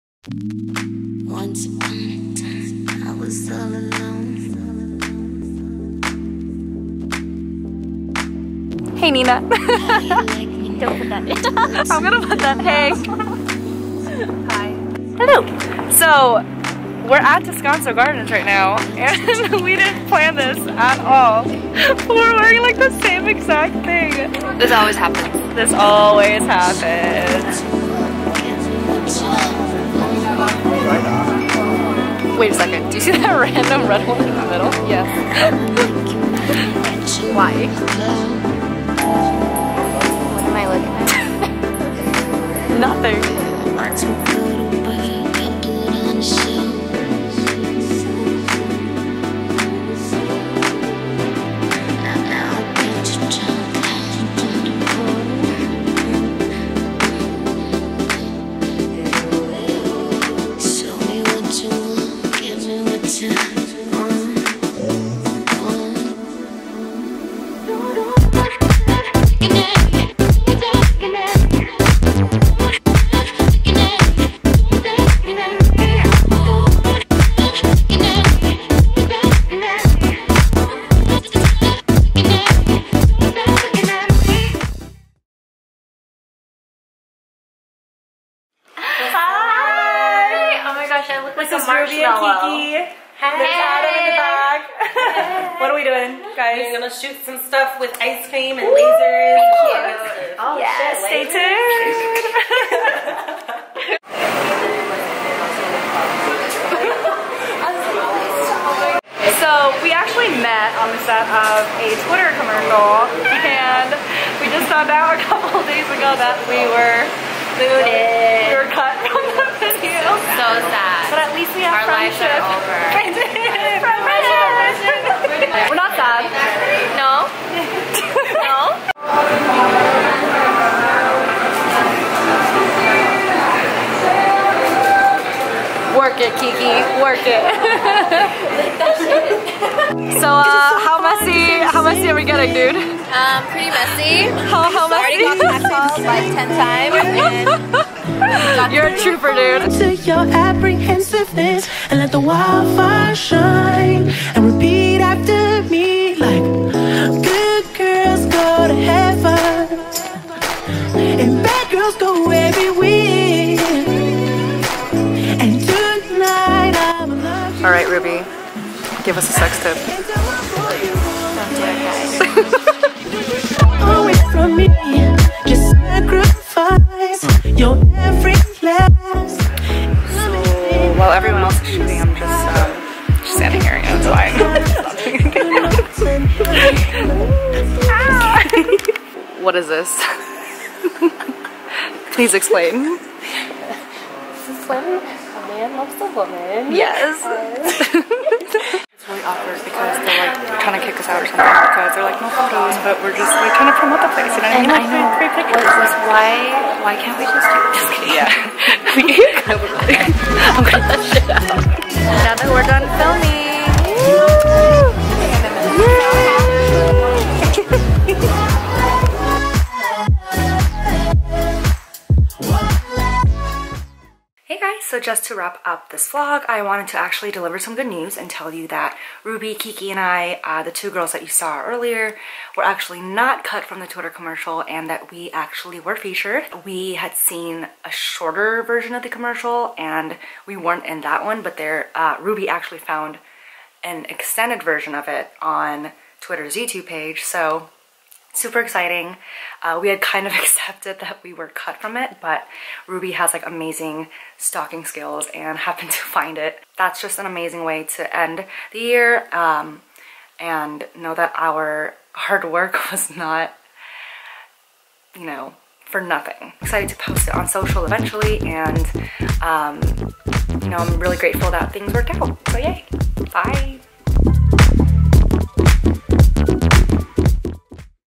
Asian Instagram Models?? // VLOG #8
In this vlog, I head on over to see some pretty holiday lights at the Descanso Gardens near Pasadena with my friends, Laura and Nina. They were pretty and holiday-ish.

THEN, I do a photoshoot with my lovely actor/social media superstar friends, Kiki Wong (@kikiwongo) and Ruby Park (@therubyelement) for, you guessed it, INSTAGRAM. We reveal some not-so-good news only to find out that we have GREAT news after all!!

Shout out to Adam Fujioka (@ninja.af) for shooting us with his amazing photography skills :D